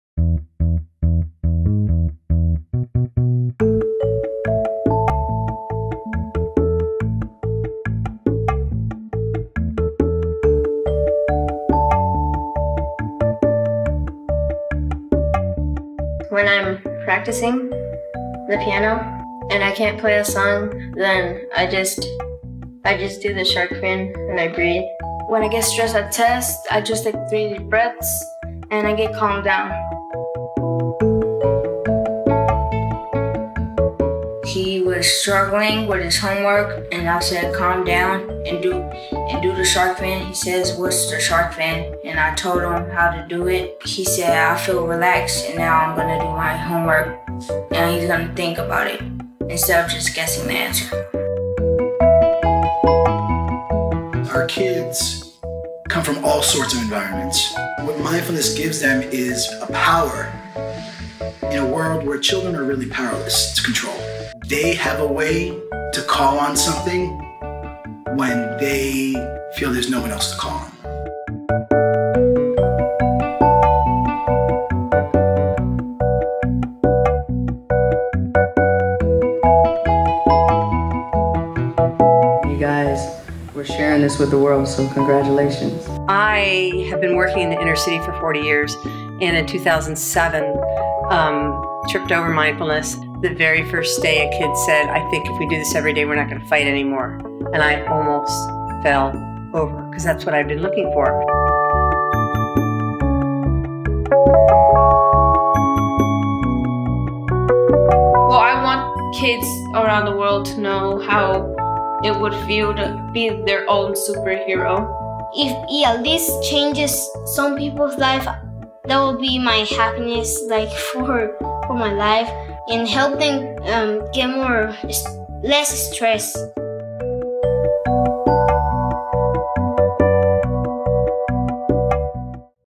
Master of Mindfulness: How to Be Your Own Superhero in Times of Stress
The children of Reach Academy in East Oakland celebrate the launch of Master of Mindfulness written by Mr. Musumeci's fifth grade class, co-written by the founder of Mindful Schools, Laurie Grossman, and illustrated by local artist Angelina Alvarez.

The book, released by New Harbinger Publications on March 1, 2016, shows kids how they can use mindfulness every day to beat stress, focus better in school, get along with others, and be their own superheroes!